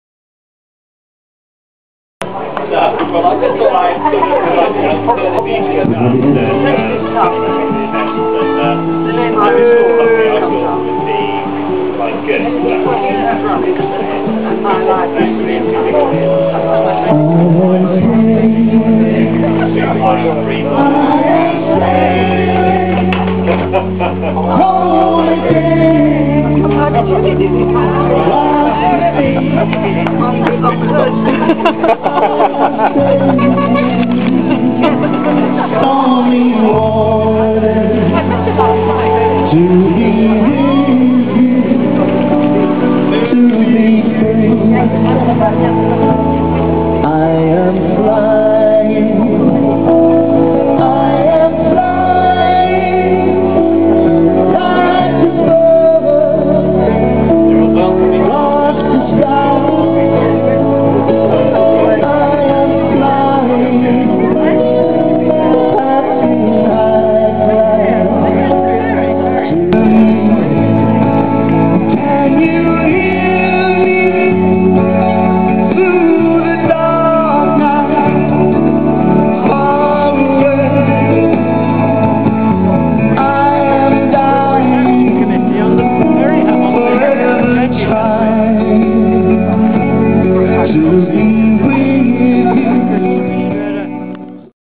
Ferry Arrives June 2012
Anniversary ferry service brings first passengers across to celebrations at The Anchor, Rowhegde, Essex.